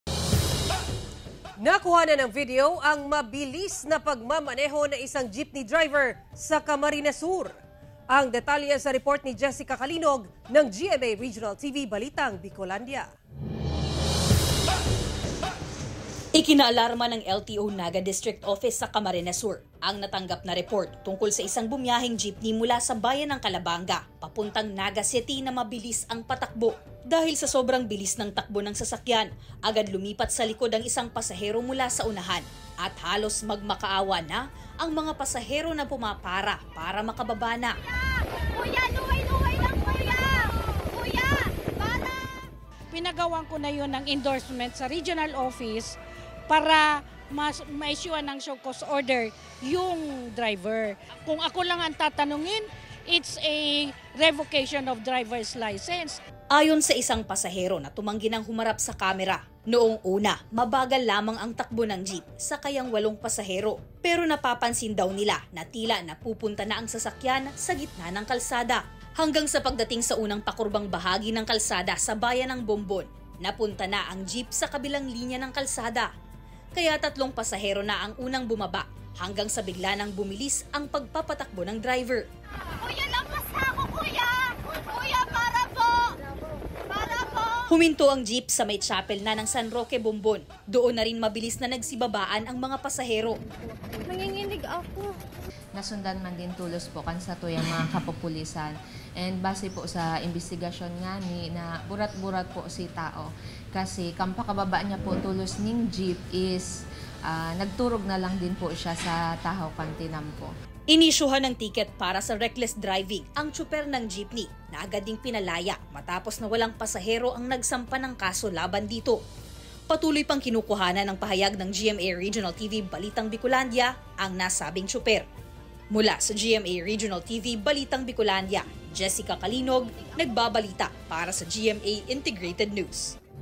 Regional TV News: Mabilis na pagmamaneho ng jeepney driver sa Camarines Sur, nakuhanan ng video
Mabilis na pagmamaneho ng isang jeepney driver sa Camarines Sur, nakuhanan ng video.